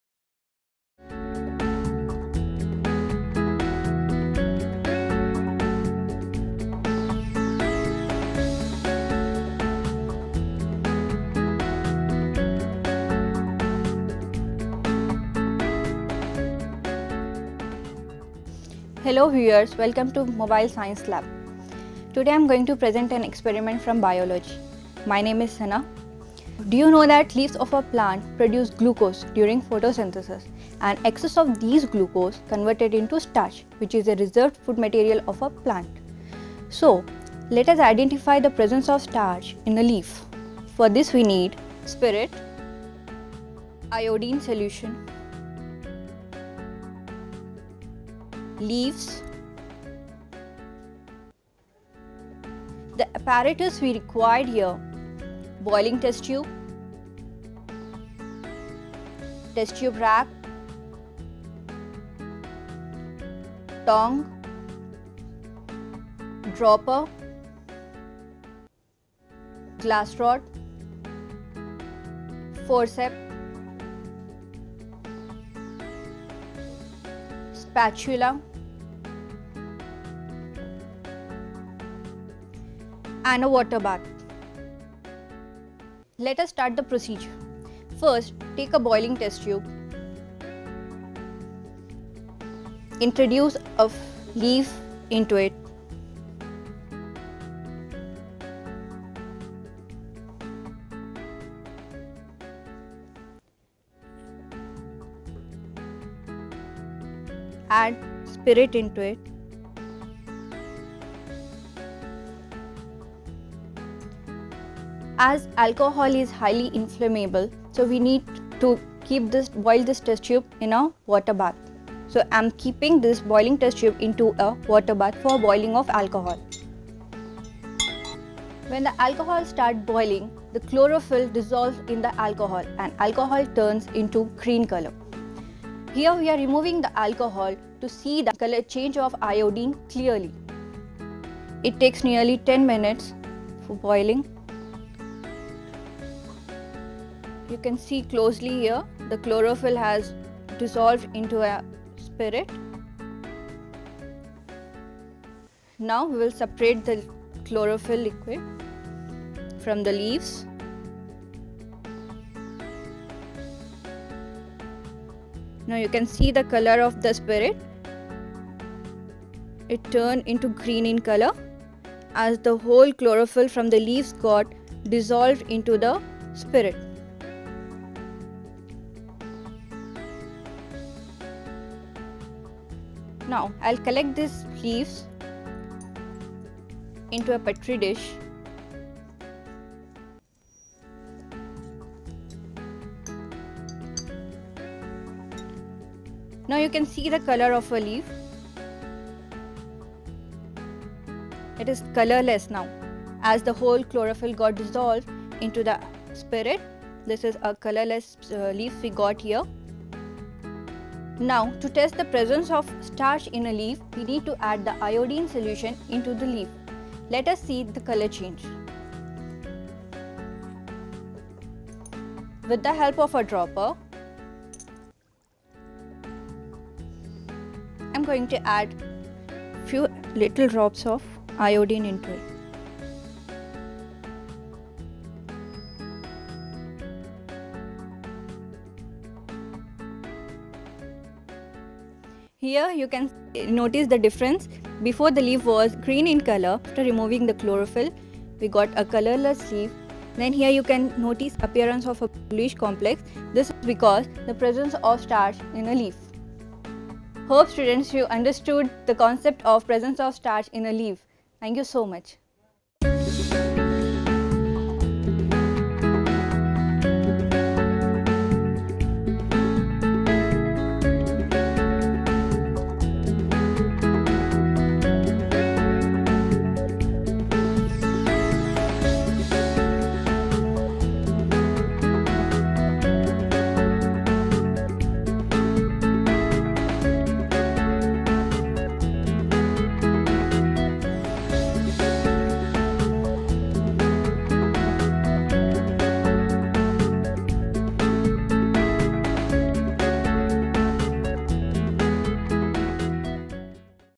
Photosynthesis
Mobile Science Lab Team explains the process of Photosynthesis